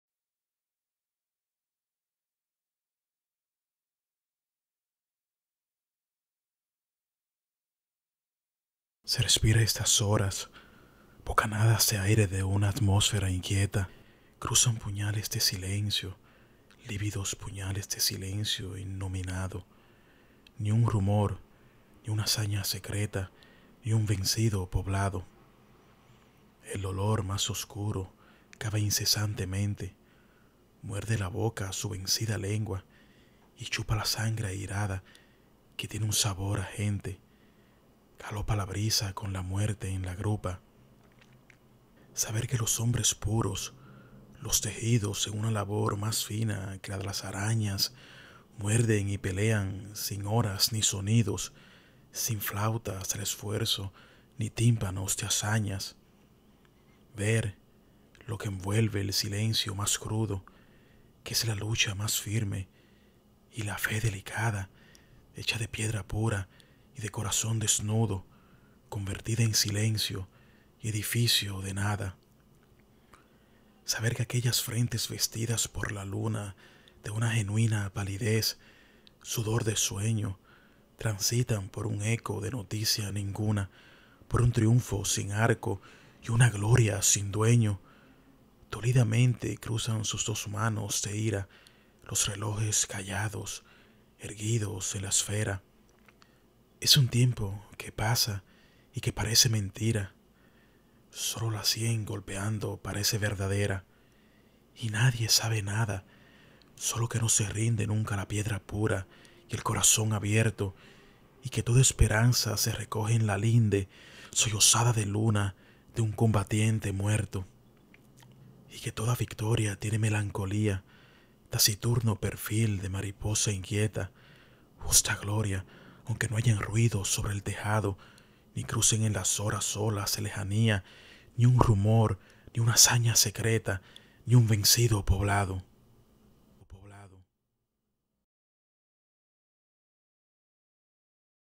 Se respira a estas horas-Pedro Mir
Se respira a estas horas
bocanadas de aire de una atmósfera inquieta.
Cruzan puñales de silencio, lívidos
puñales de silencio innominado.
Ni un rumor, ni una hazaña secreta,
ni un vencido poblado.

El dolor más oscuro cava incesantemente.
Muerde la boca su vencida lengua, y chupa
la sangre airada que tiene un sabor a gente.
Galopa la brisa con la muerte en la grupa.

Saber que los hombres puros, los tejidos
en una labor más fina que la de las arañas,
muerden y pelean sin horas ni sonidos,
sin flautas del esfuerzo ni tímpanos de hazañas.

Ver lo que envuelve el silencio más crudo.
Que es la lucha más firme y la fe delicada,
hecha de piedra pura y de corazón desnudo,
convertida en silencio y edificio de nada.

Saber que aquellas frentes vestidas por la luna
de una genuina palidez, sudor de sueño,
transitan por un eco de noticia ninguna,
por un triunfo sin arco y una gloria sin dueño.

Dolidamente cruzan sus dos manos de ira
los relojes callados, erguidos en la esfera.
Es un tiempo que pasa y que parece mentira.
Sólo la sien golpeando parece verdadera.

Y nadie sabe nada, sólo que no se rinde
nunca la piedra pura y el corazón abierto.
Y que toda esperanza se recoge en la linde
sollozada de luna de un combatiente muerto.

Y que toda victoria tiene melancolía.
Taciturno perfil de mariposa inquieta.
Justa gloria, aunque no hayan ruidos sobre el tejado.
Ni crucen en las horas solas de lejanía,
ni un vencido poblado. Pedro Mir